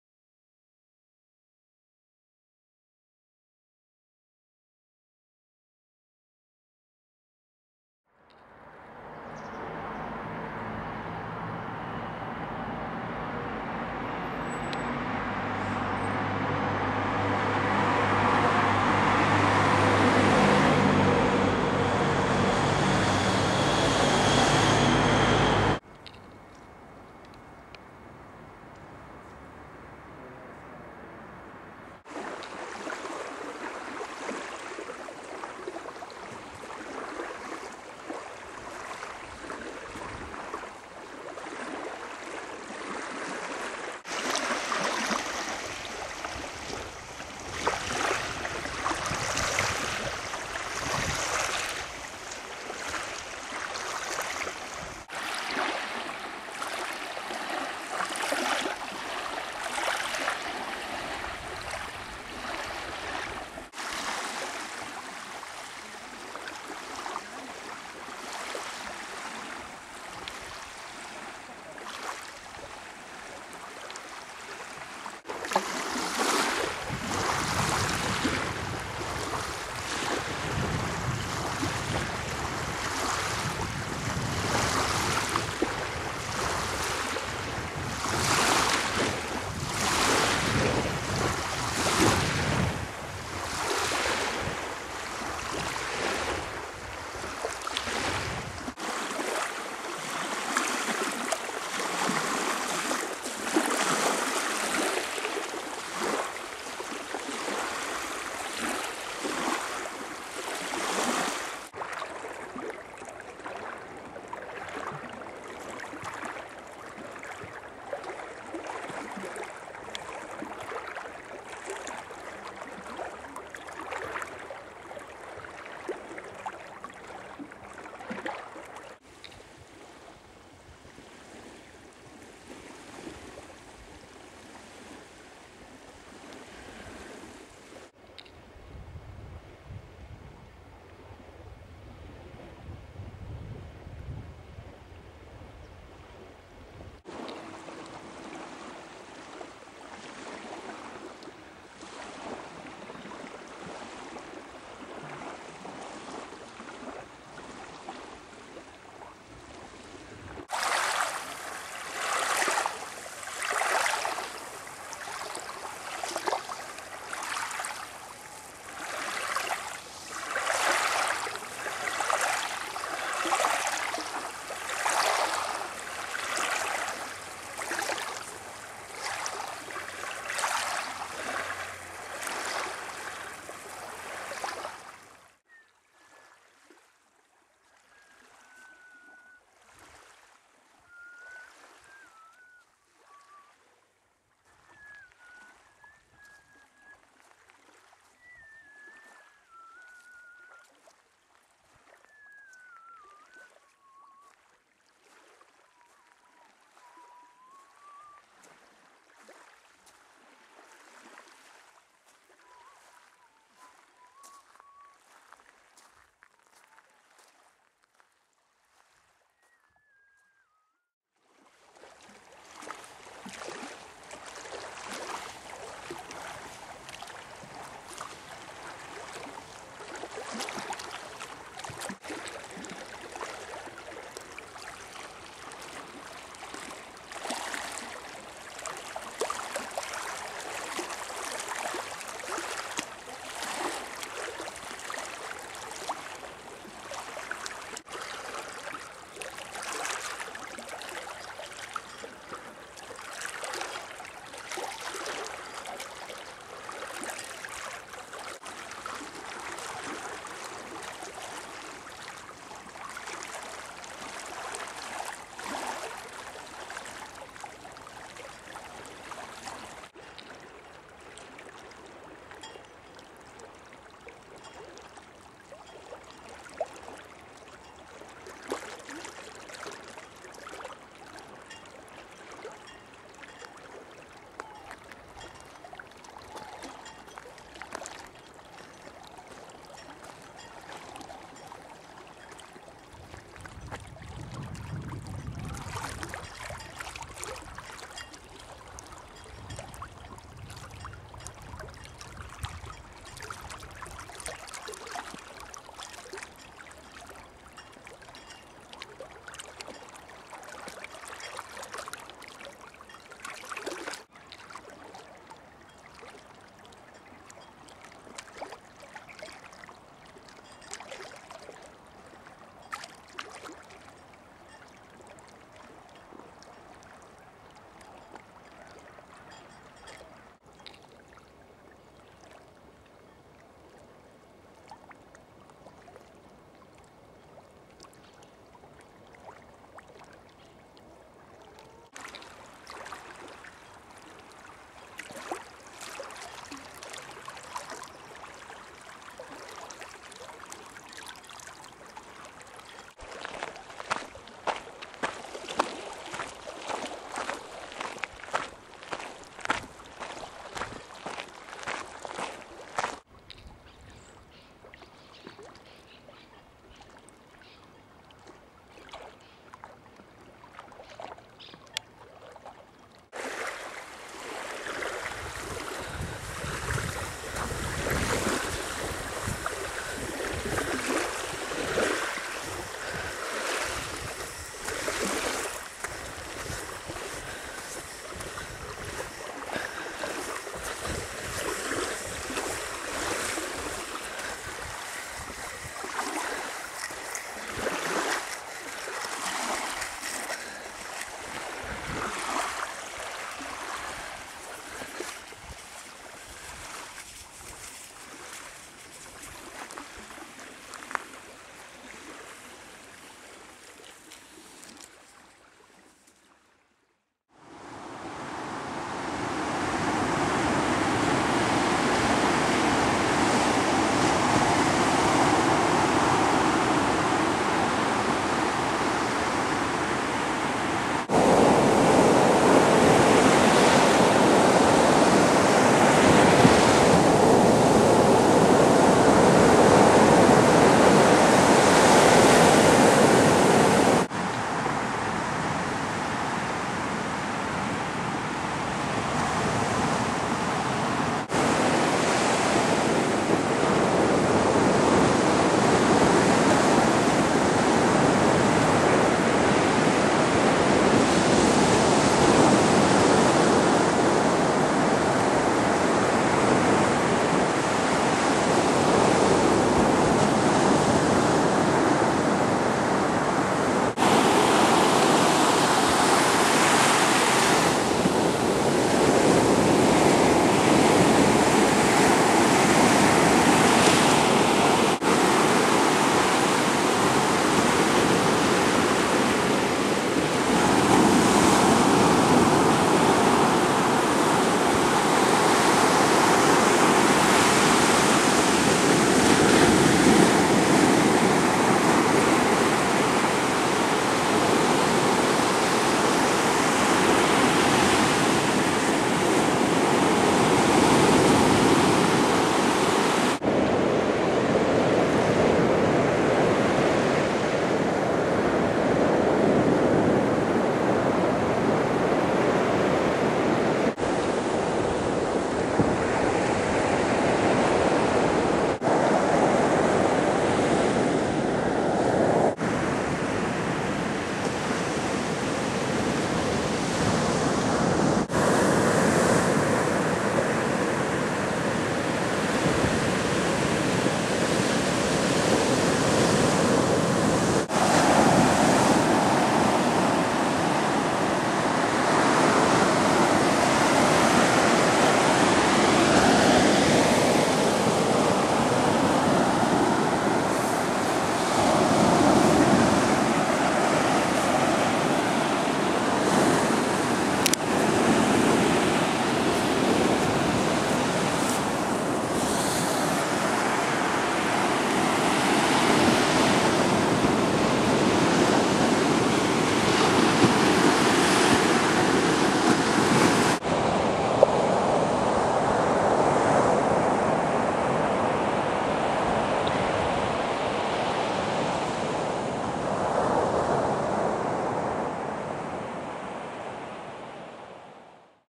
"El rastro" Video Arte artista murciana AtlanLy - Mar Menor
Video Arte
Artista: Lydia Olivares
Proyección y vídeo creación por la artista AtlanLy. Muestra una queja "ahogada" de la situación del mar menor, en la región murciana.

La pieza artística fué llevada a cabo en el año 2010.